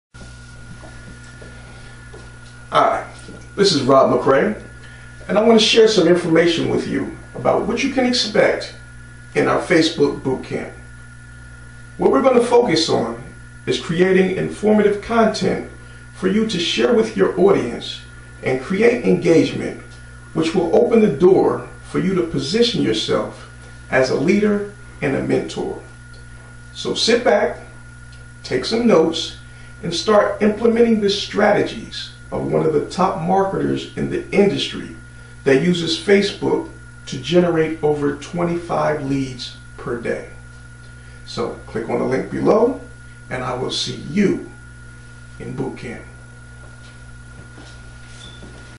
RM facebook Bootcamp
Facebook Marketing Bootcamp

Learn how to leverage the power of Facebook

Great training on how to generate multiple leads per day via facebook.

Once you master these strategies you can create a massive amount of leads and sales with any business.

Facebook is a goldmine and it is designed for business owners that understand how to use the site and follow their guidelines so take notes and become the next facebook master marketer.

Rob Mccray